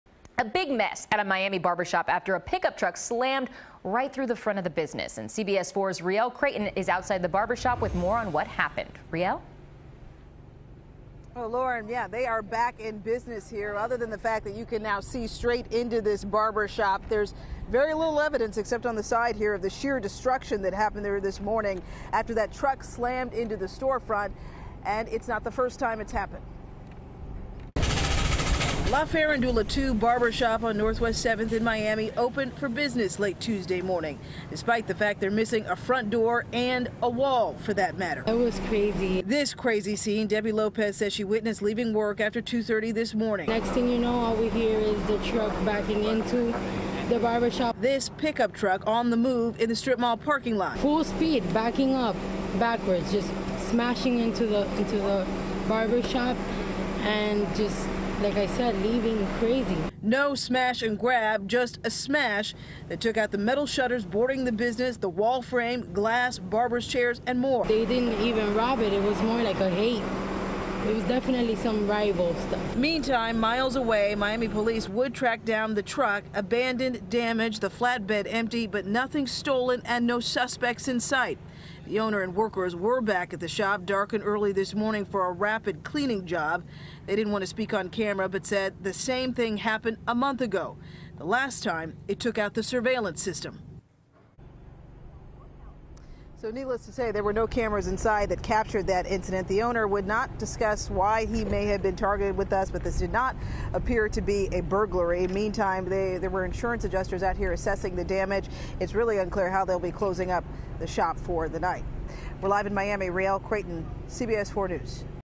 Truck Slams Into Front Of Miami Barbershop
An investigation is underway after a pickup truck slammed into the front of a Miami barbershop overnight.